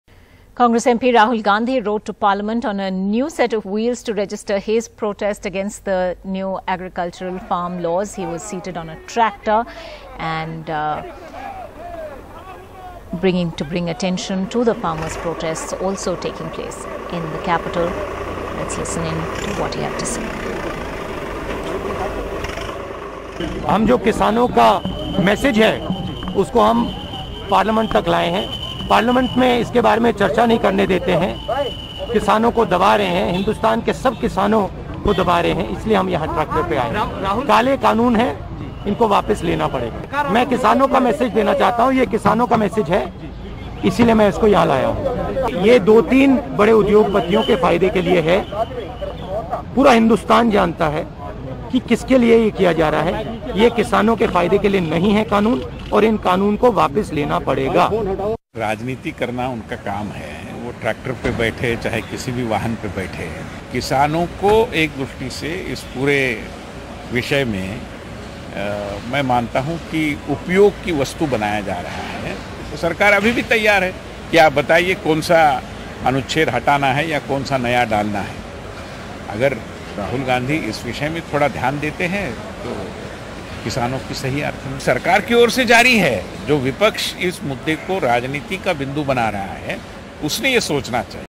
"Tractor In Parliament": Rahul Gandhi Warns Government Over Farm Laws
Rahul Gandhi today rode a new set of wheels to Parliament carrying a warning, farmers' message and registering his protest against the three agricultural laws. Seated at the wheel on a blazing red tractor, Rahul Gandhi, sporting a half-sleeved shirt, trousers and sandals, rode along confidently with a face mask on in the heart of Delhi, marking his protest against the controversial farm laws.